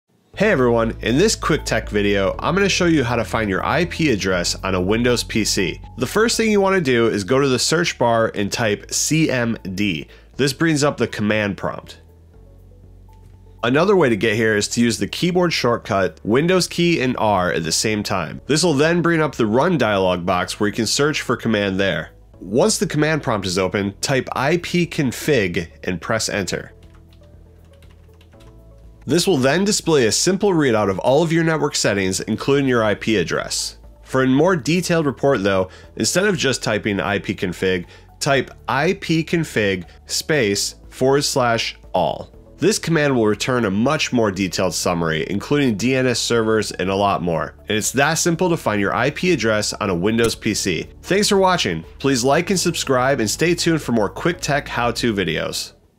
How To Find Your IP Address On A Windows 10 PC
In this quick video, I’m going to show you how to find your IP address on a Windows PC.

The first thing you want to do, is go the search bar and type, CMD. This brings up the Command Prompt.

Another way to get here is to us the keyboard shortcut Windows key and R at the same time. This will then bring up the Run dialogue where you can search Command, or CMD, from there.

Once the Command Prompt is open, type, IPCONFIG and press Enter.

This will then display a simple readout of all of your Network settings, including your IP Address.

For a more detailed report, instead of just typing IPCONFIG, type, IPCONFIG /ALL.

This will return a much more detailed summary, including DNS Servers and more.

And it's that simple to find your IP Address on a Windows PC.